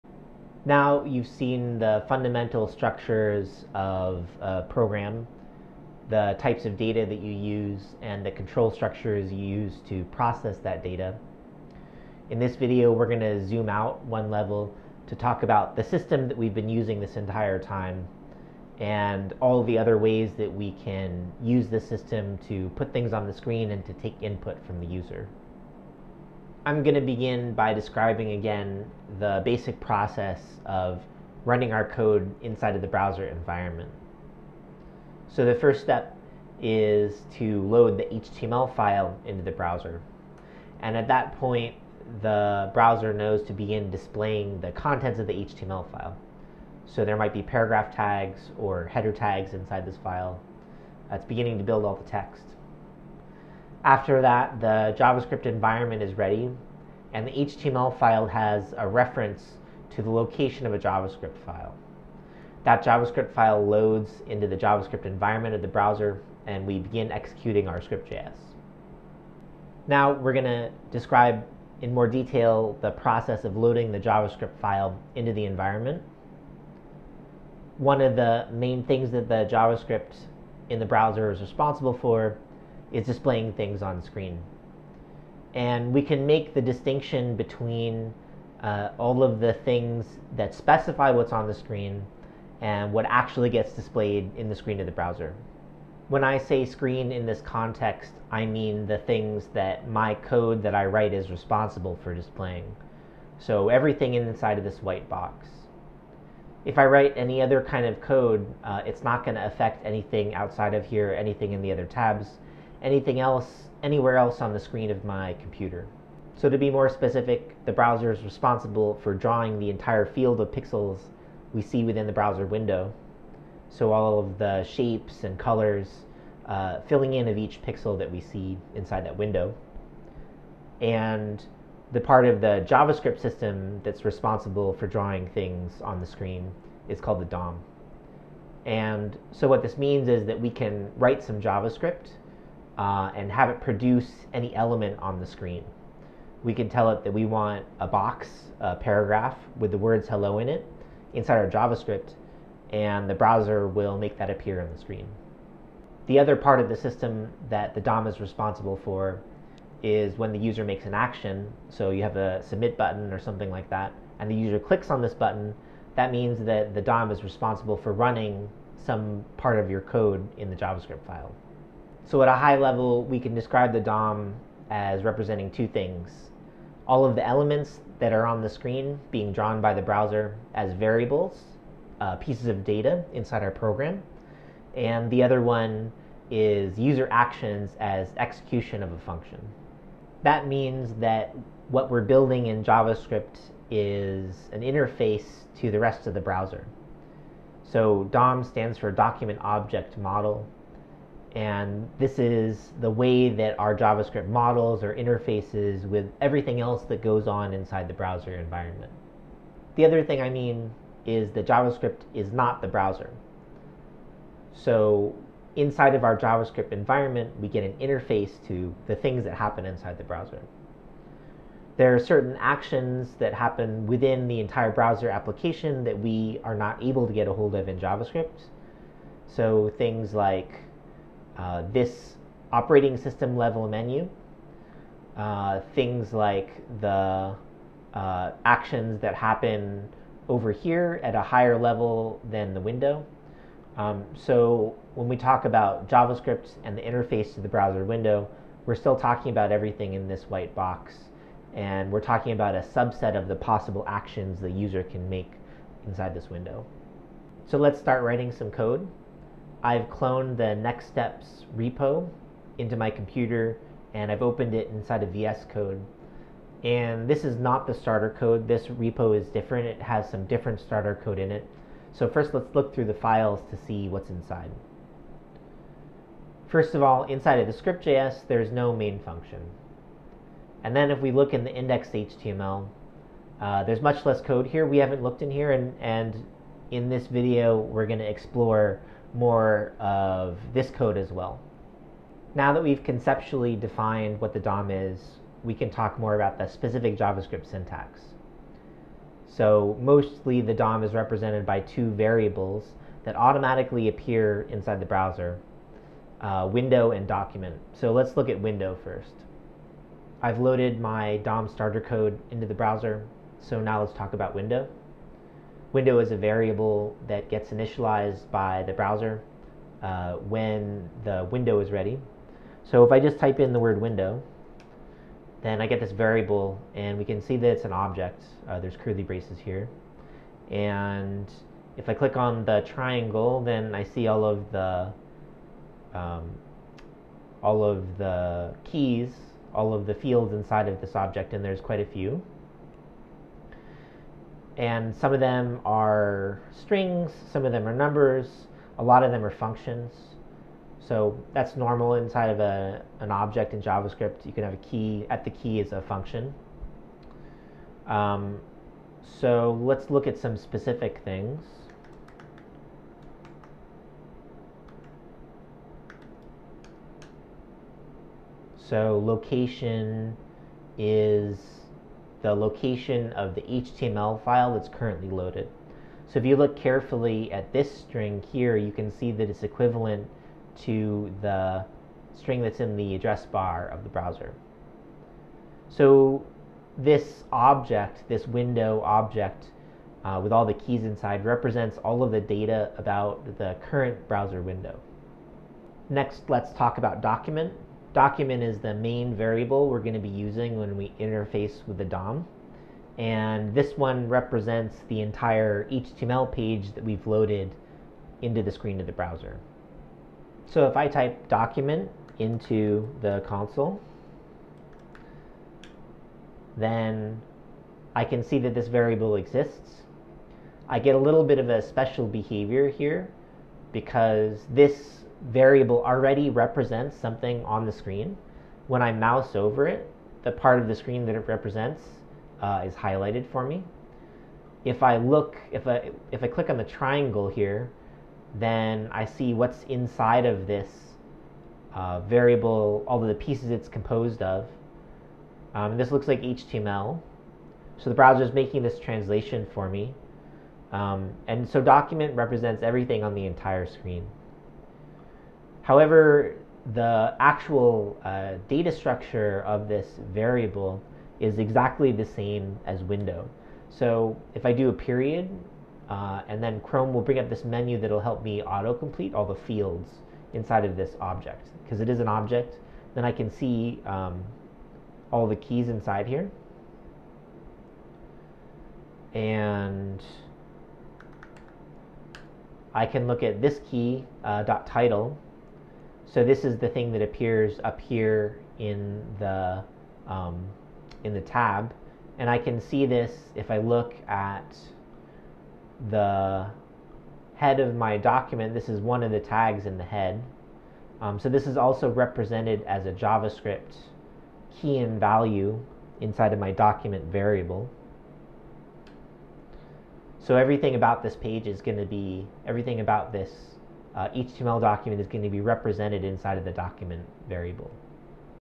Basics: DOM - Intro
The Document Object Model (DOM) is a representation of HTML that can be manipulated using JavaScript. Each element in the HTML can be accessed as a variable in JavaScript. User actions on those HTML elements can also trigger code logic in JavaScript. The DOM also exposes 2 global JavaScript objects `window` and `document` that allow developers to manipulate properties of the browser window and HTML element tree respectively.

Get a software engineering job with Rocket Academy. Job success or your money back. Singapore's most comprehensive coding bootcamp course.